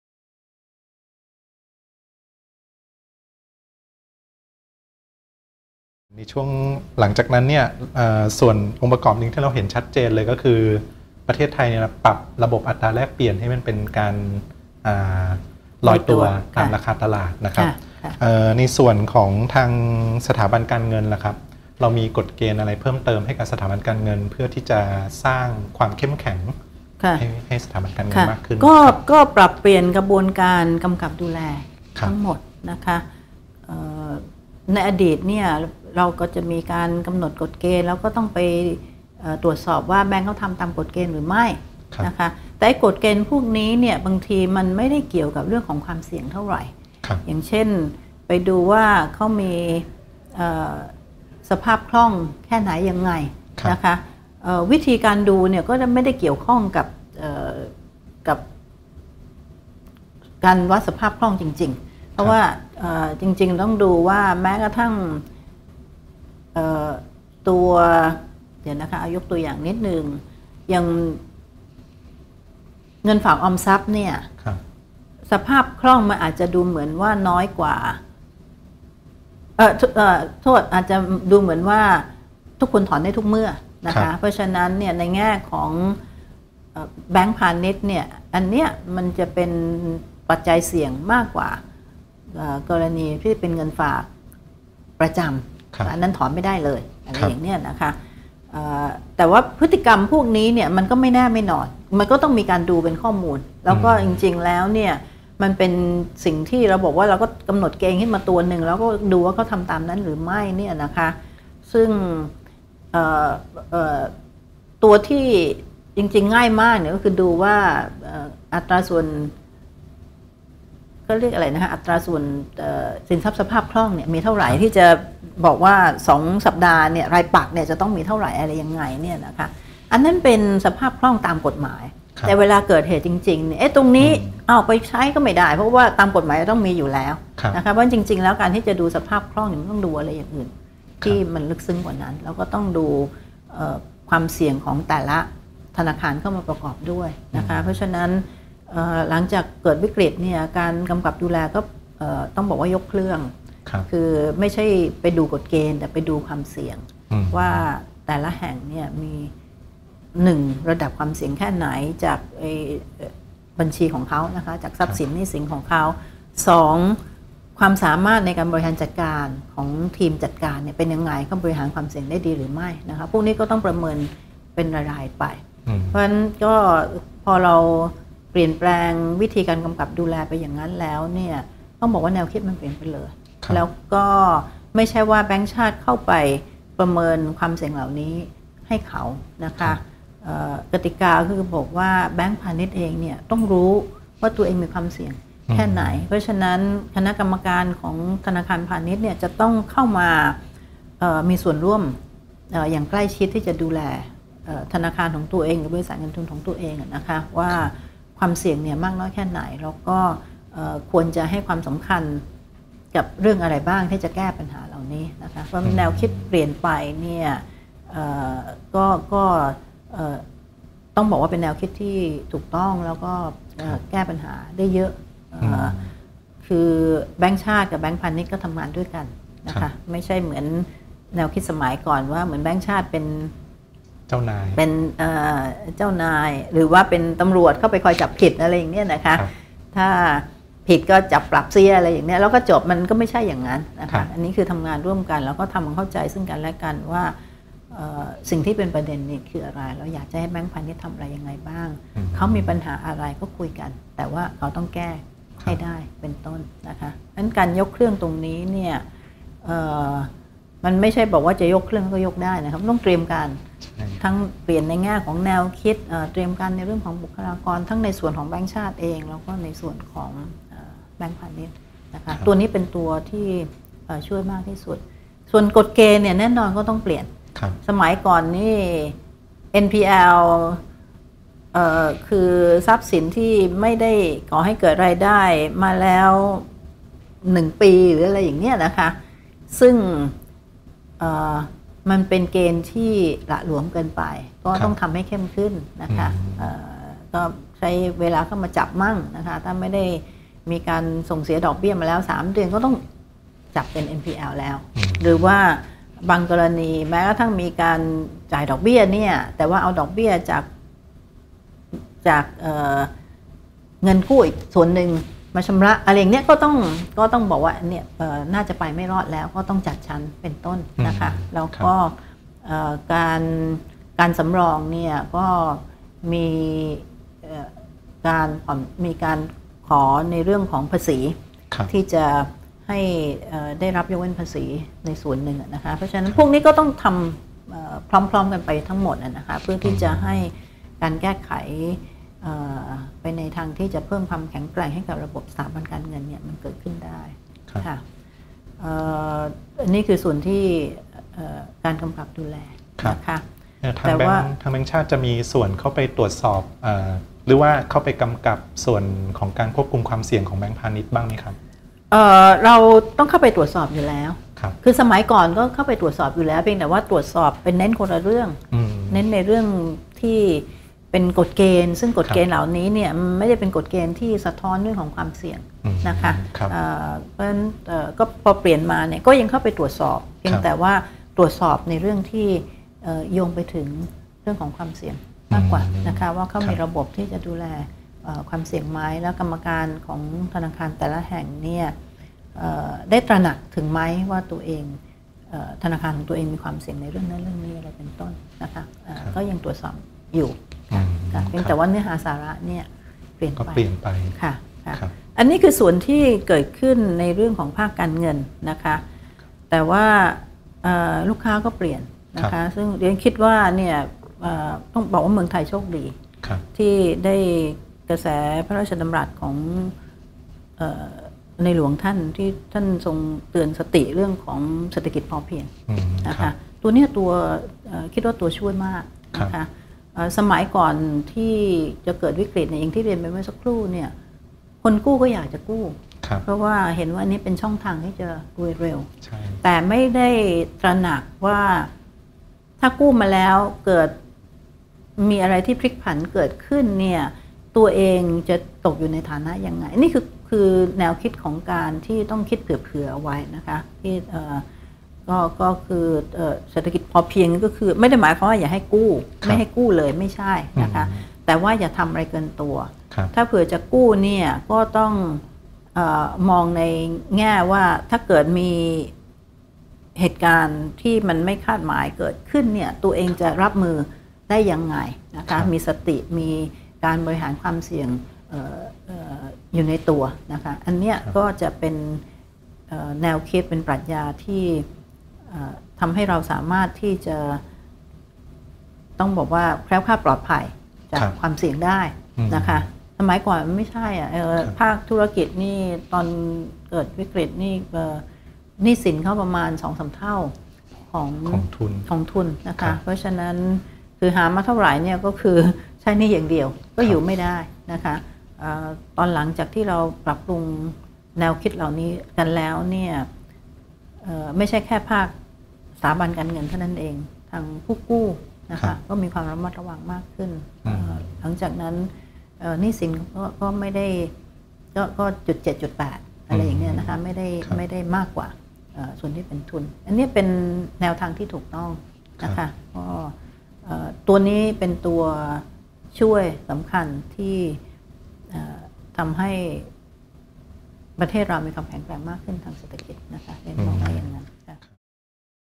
ซีรีส์ "15 ปี วิกฤติ 2540" ธาริษา วัฒนเกส 6/11
หลังวิกฤติ 2540 นอกจาก ธปท.จะปรับเปลี่ยนระบบอัตราแลกเปลี่ยน และหันมาใช้นโยบายกำหนดเป้าหมายเงินเฟ้อ หรือ Inflation targeting แล้ว ธปท. ยังได้ปฏิรูประบบการกำกับและตรวจสอบสถาบันการเงินครั้งใหญ่ โดยเฉพาะบทบาทของ ธปท.จากเดิมที่ชอบทำตัวเป็นตำรวจที่คอยเข้าไปจับผิด มาเป็นการทำงานร่วมกันกับสถาบันการเงิน  ตอนนั้น ธปท.ทำอะไรบ้าง และสถาบันการเงินปรับตัวให้เข้มแข็งได้อย่างไร ติดตามได้จากบทสัมภาษณ์พิเศษ "ดร.ธาริษา วัฒนเกส" ตอนที่ 6